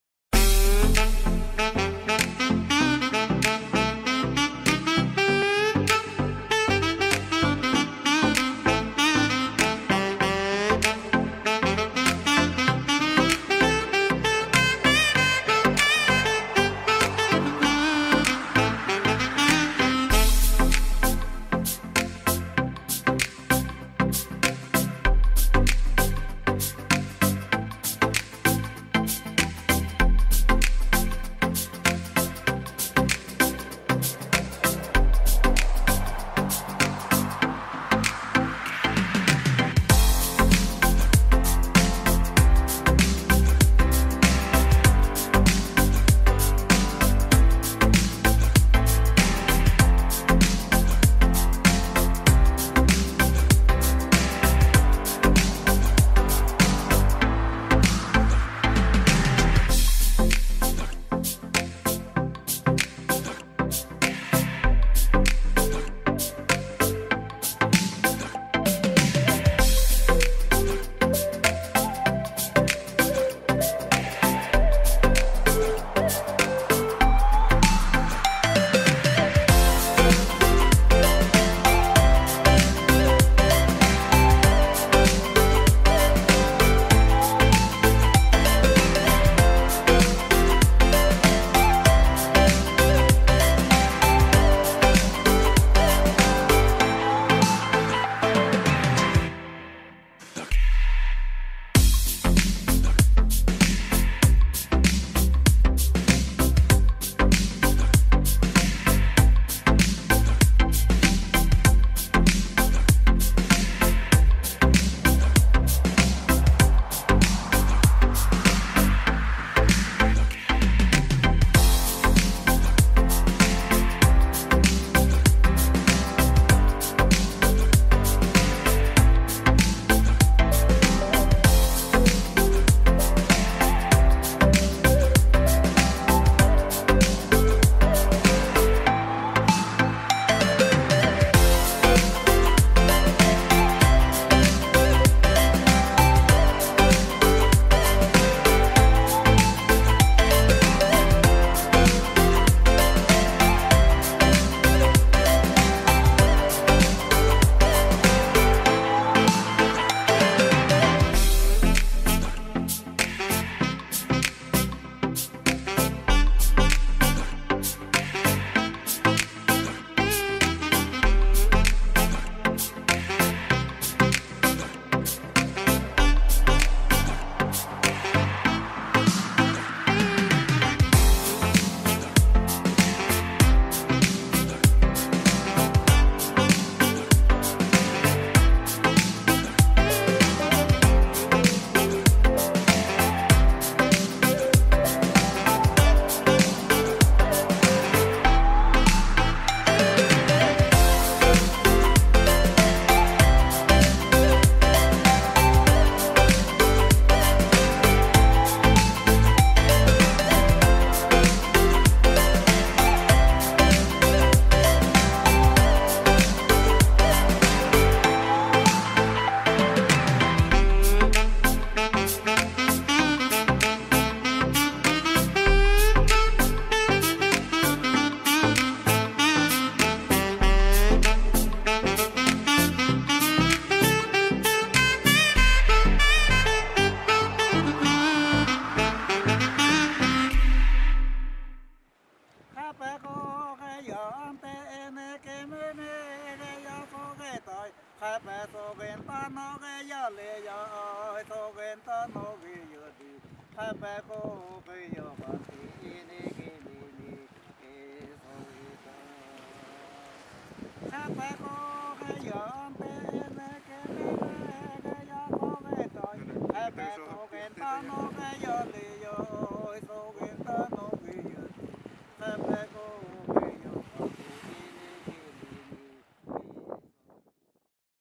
Gulf of California
Spring 2016 Duke University Marine Lab class trip to Sonora, Mexico with Dr. Xavier Basurto. Thanks to Donovan, Victoria, Connie and Xavier for contributing footage.